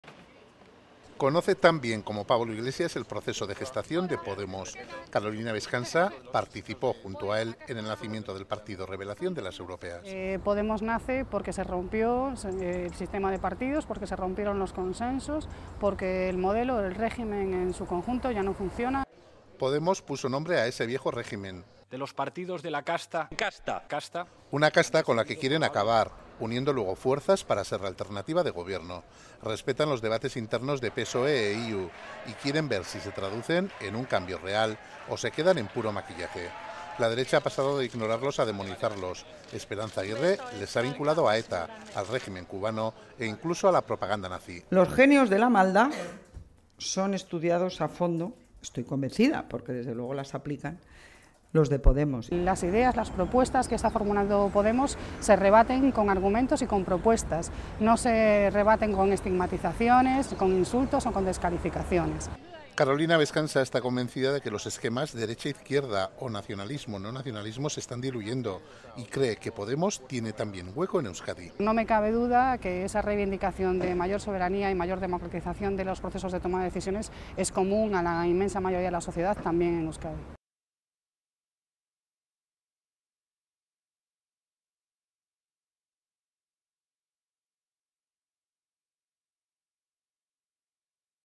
Carolina Bescansa (Podemos) nos explica el fenómeno Podemos
Conoce tan bien como Pablo Iglesias el proceso de gestación de Podemos. Carolina Bescansa participó junto a él en el nacimiento del partido revelación de las europeas.